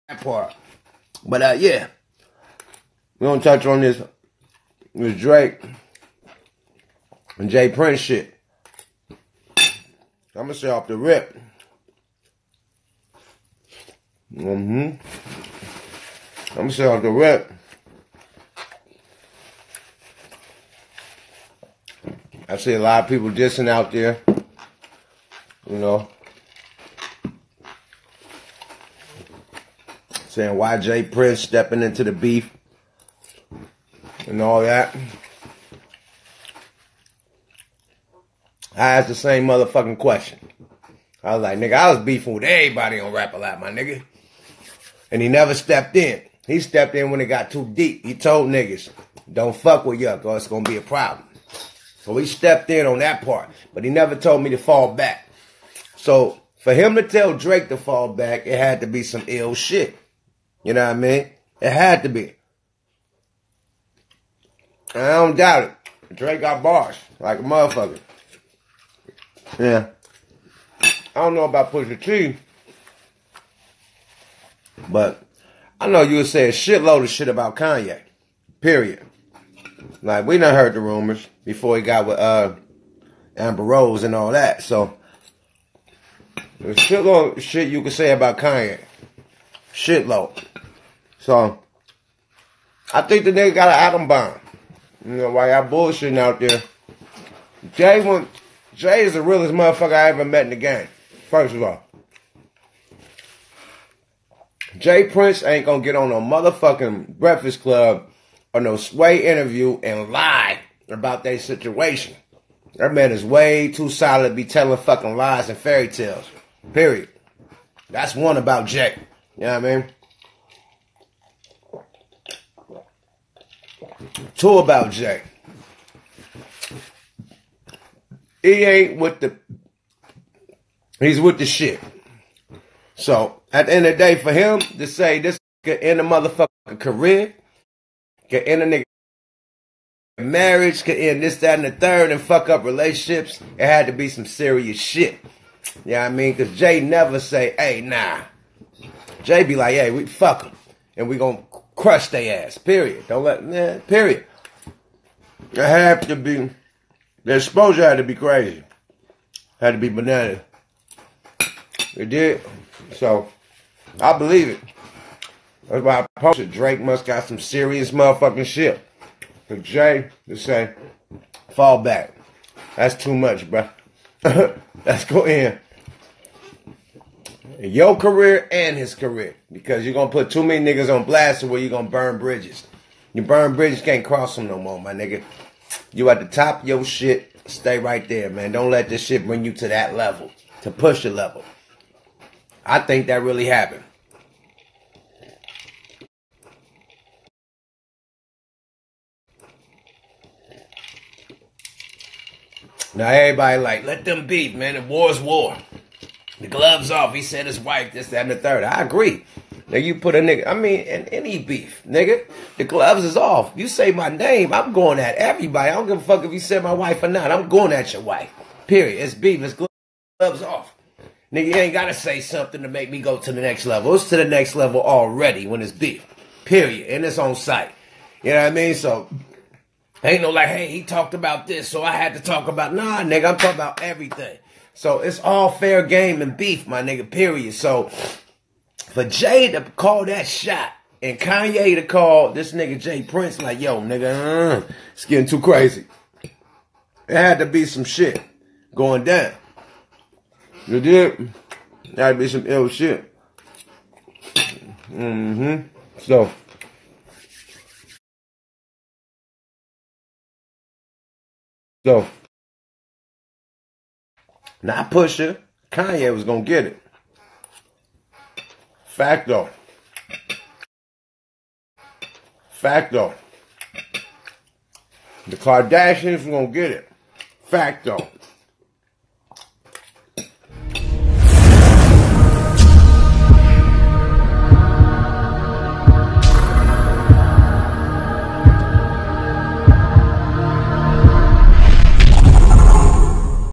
Yukmouth Speaks on J Prince and the Drake x Pusha T Beef [BayAreaCompass]
Yukmouth has gone thru plenty battles with J Prince on his side but Speaks on why J Prince stepped in on the situation